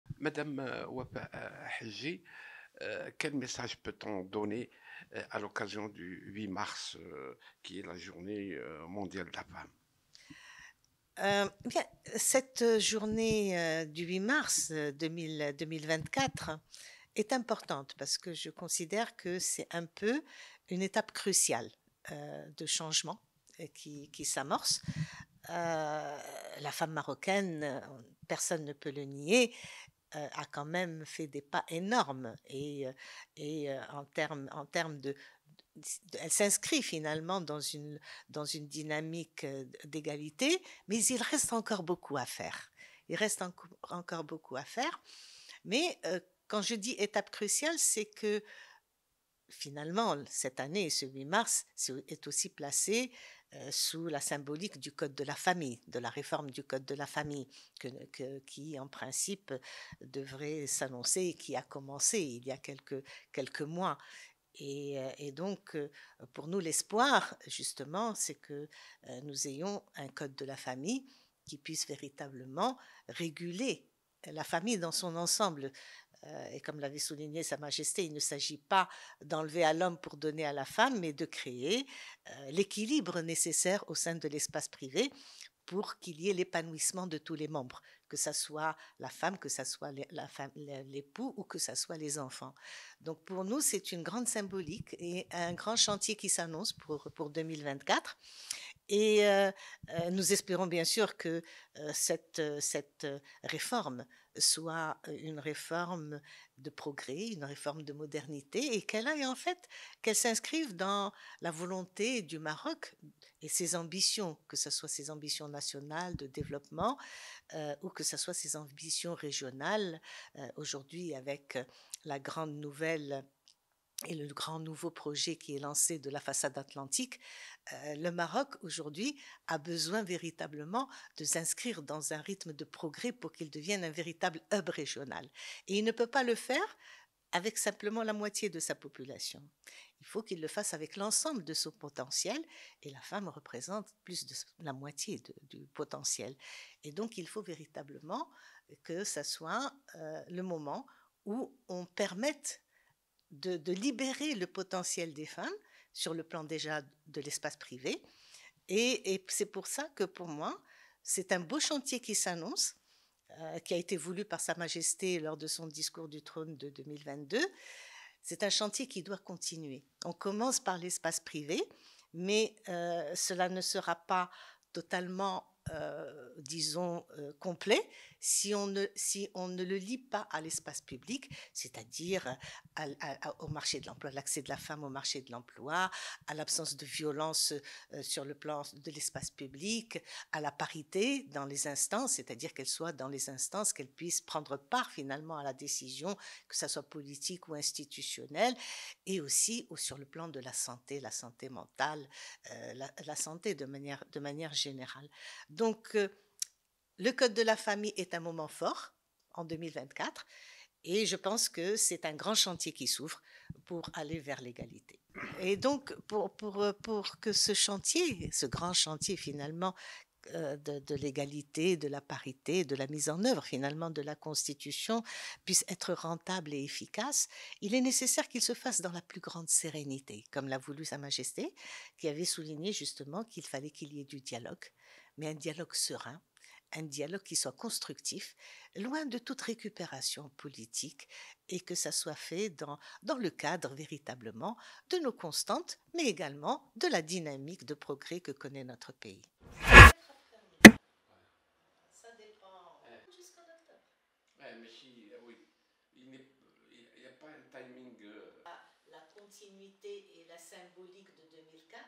8 mars: “il y a des avancées mais il reste beaucoup à faire » pour la femme, awlon Ouafa Hajji
À l’occasion de la célébration de la Journée internationale des droits des femmes, ce vendredi 8 mars, Ouafa Hajji, coordinatrice du collectif Parité maintenant, estime que si les droits des femmes se sont améliorés au Maroc, «il reste encore beaucoup à faire dans ce domaine».